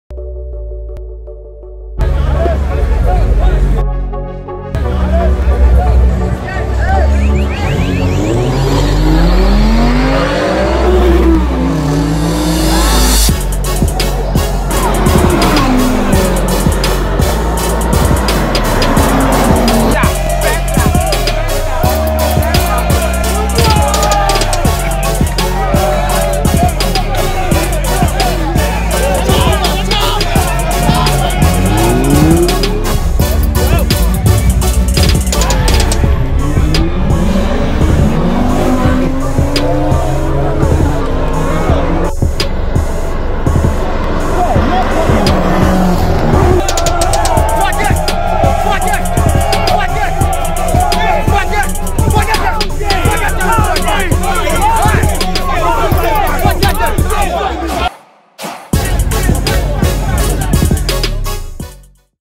Lamborghini Huracan vs BMW M5 || Drag race
Drag race footage from the Accra Launch Control event in Ghana organized by the Auto-motorsports Community and BMW Club .